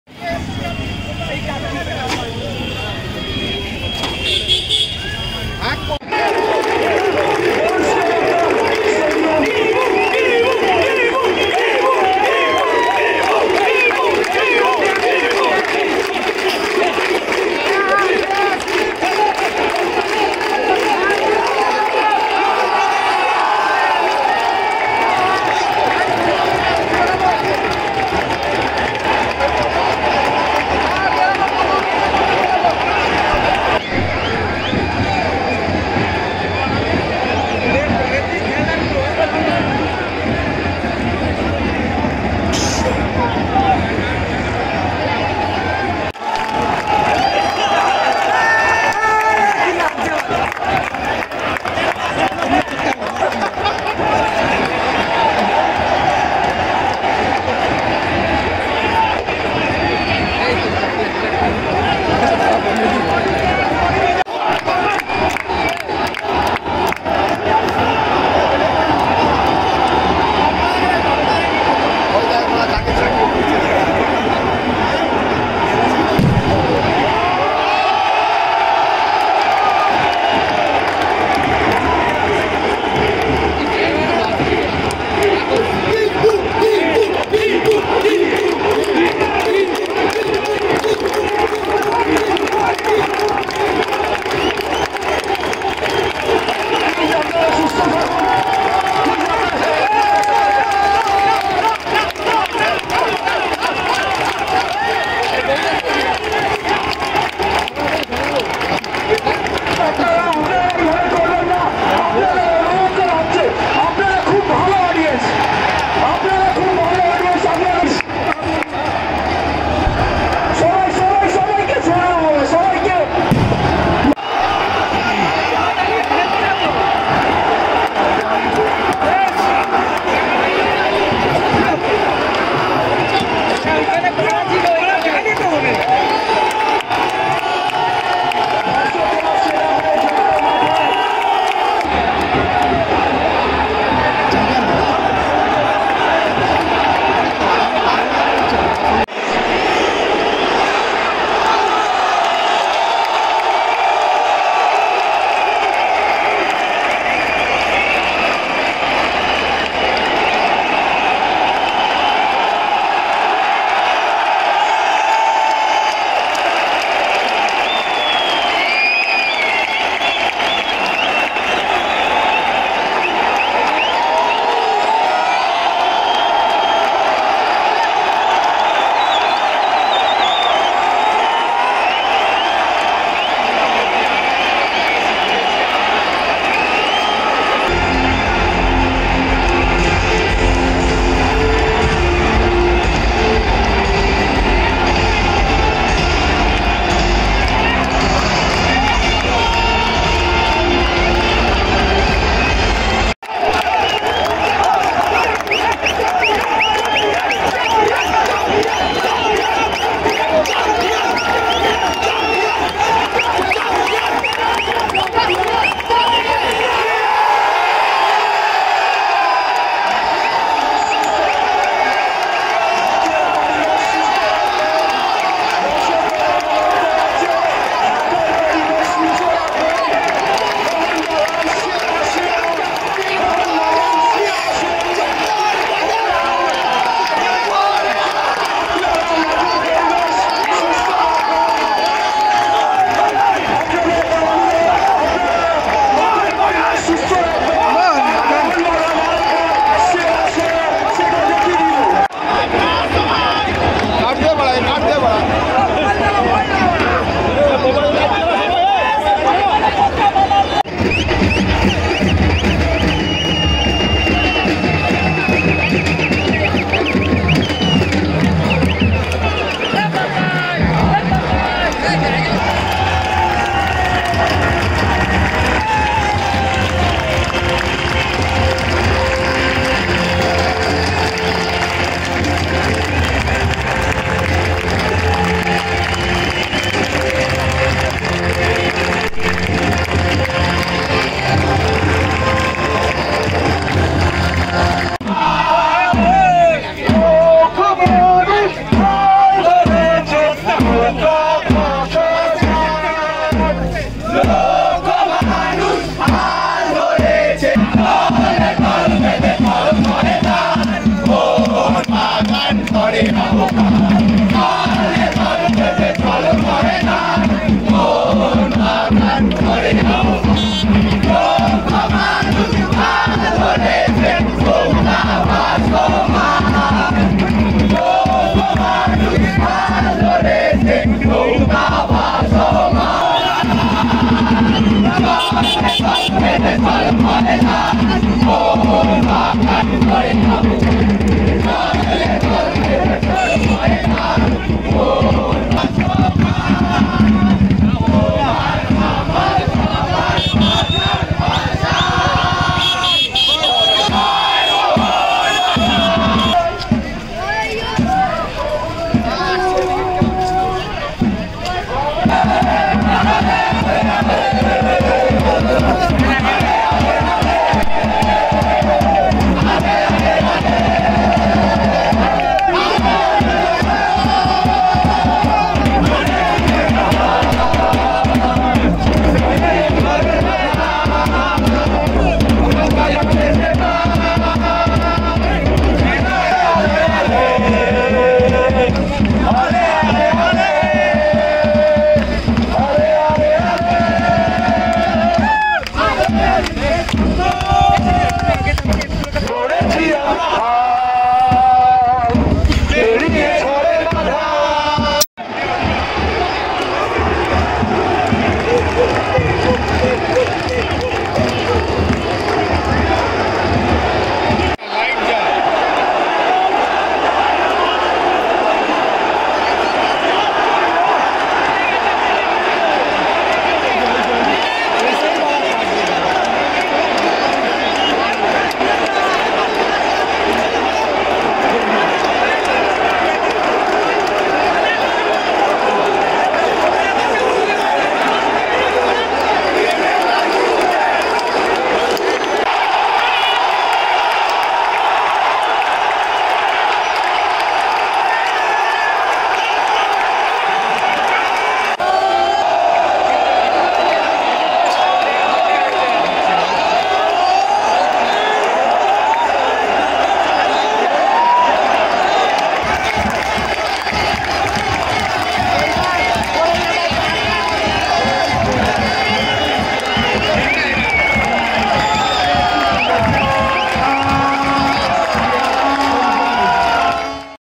Mohun Bagan i league winning celebration and fan craze | i league 2019-20 |winner of i league 2019
hi friends i think you all are stay safe. in this lockdown we all are bore so i share this beautiful moments of mohun bagan fans. i think you all enjoy it,